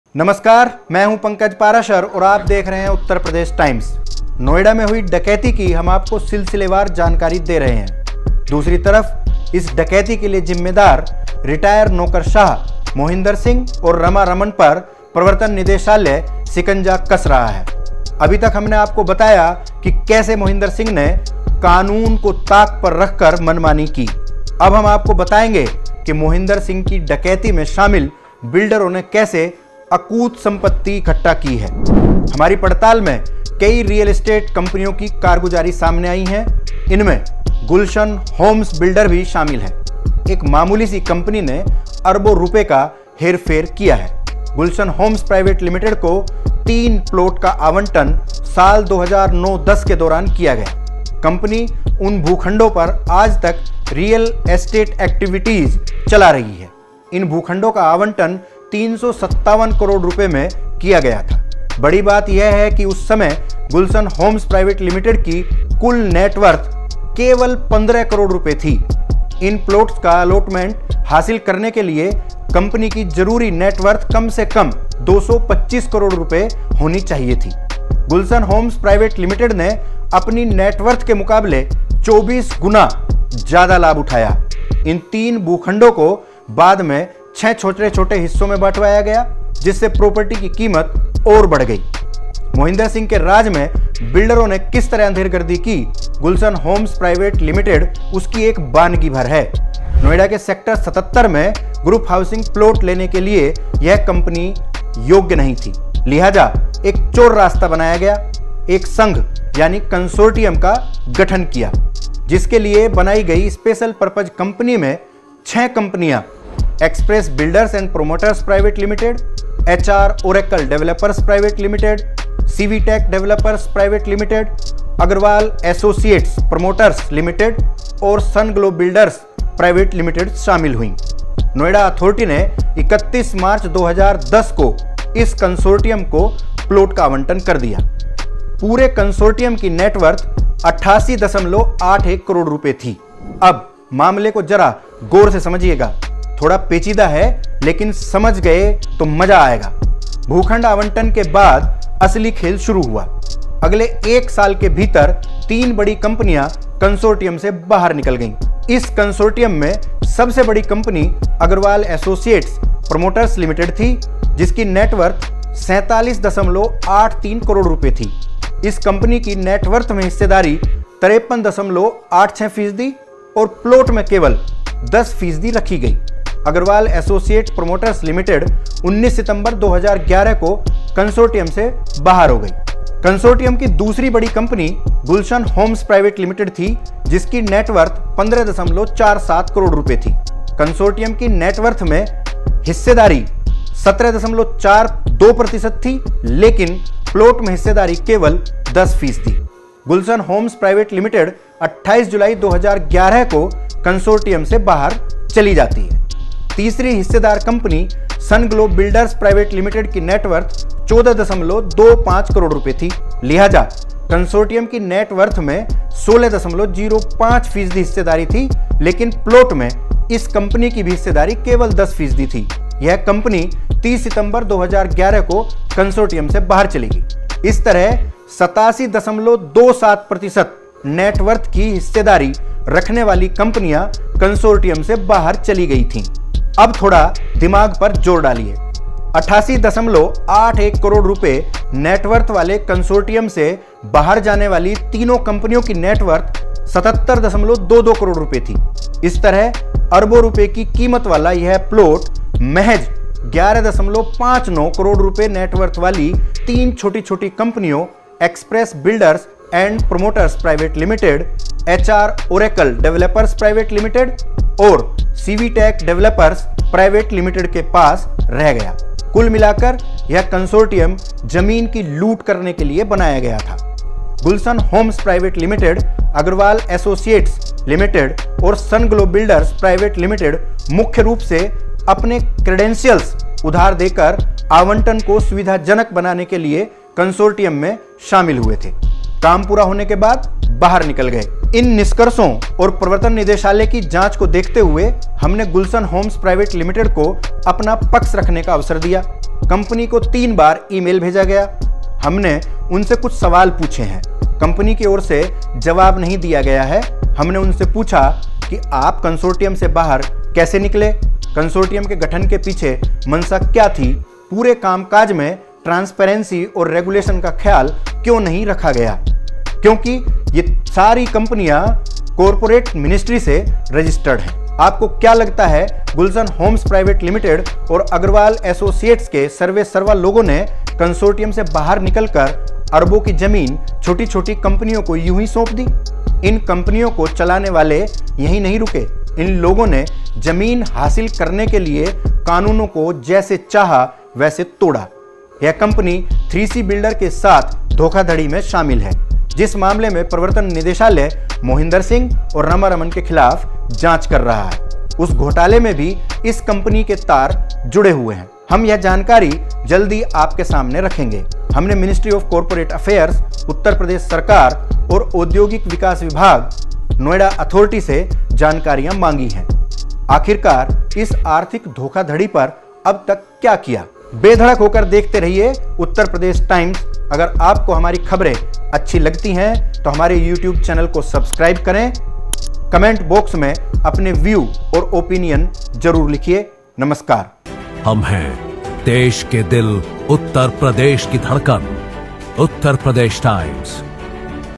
Noida की लूट कथा | Mohinder मेहरबान तो बिल्डर मालामाल | Real State कंपनियों की कारगुजारी सामने आई
नोएडा में हुई डकैती की हम आपको सिलसिलेवार जानकारी दे रहे हैं। दूसरी तरफ इस डकैती के लिए जिम्मेदार रिटायर नौकरशाह मोहिंदर सिंह और रमा रमन पर प्रवर्तन निदेशालय शिकंजा कस रहा है। अभी तक हमने आपको बताया कि कैसे मोहिंदर सिंह ने कानून को ताक पर रखकर मनमानी की। अब हम आपको बताएंगे कि 'मोहिंदर सिंह की डकैती' में शामिल बिल्डरों ने कैसे अकूत संपत्ति इकठ्ठा की है।

हमारी पड़ताल में कई रियल एस्टेट कंपनियों की कारगुजारी सामने आई हैं। इनमें गुलशन होम्ज़ बिल्डर भी शामिल है। एक मामूली सी कंपनी ने अरबों रुपये का हेरफेर किया है। गुलशन होम्ज़ प्राइवेट लिमिटेड को तीन प्लॉट का आवंटन वर्ष 2009-10 के दौरान किया गया। कंपनी उन भूखंडों पर आज तक रियल एस्टेट गतिविधियां संचालित कर रही है। इन भूखंडों का आवंटन 357 करोड़ रुपये में किया गया था। बड़ी बात यह है कि उस समय गुलशन होम्ज़ प्राइवेट लिमिटेड की कुल नेटवर्थ 15 करोड़ रुपये थी। इन प्लॉट्स का एलॉटमेंट हासिल करने के लिए कंपनी की जरूरी नेटवर्थ कम से कम 225 करोड़ रुपये होनी चाहिए थी। गुलशन होम्ज़ प्राइवेट लिमिटेड ने अपनी नेटवर्थ के मुकाबले 24 गुना अधिक लाभ उठाया। इन तीन भूखंडों को बाद में 6 हिस्सों में विभाजित करवाया गया था। जिससे प्रॉपर्टी की कीमत और बढ़ाई गयी।